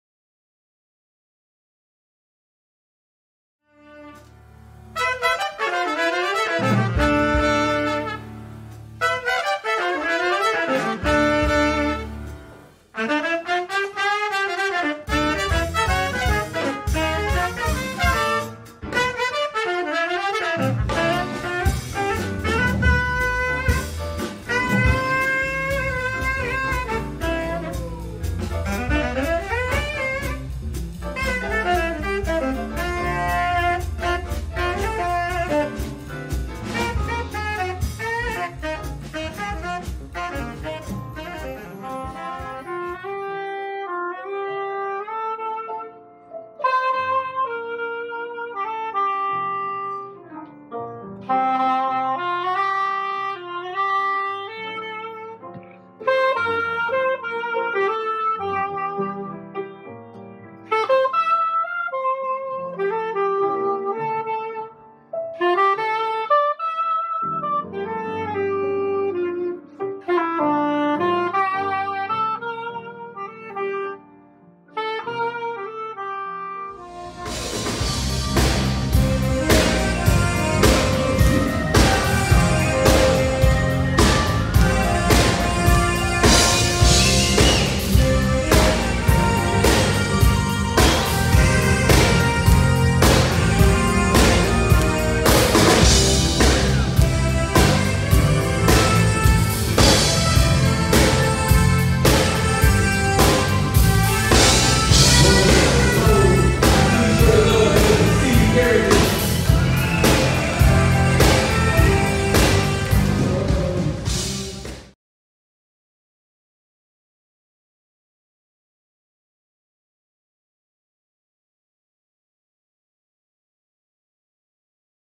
SCQUINTET EPK 3 14
STEVE CARRINGTON QUINTET/QUARTET  a.k.a SCQ
Short promo and EPK.  2022

Steve Carrington - Tenor and Soprano Saxes
Freddie Hendrix - Trumpet and Flugelhorn
Cyrus Chestnut - Piano
John Lee - Bass
Christopher Brown - Drums

Steve Carrington  w/Kool and The Gang
at their 50th anniversary European Tour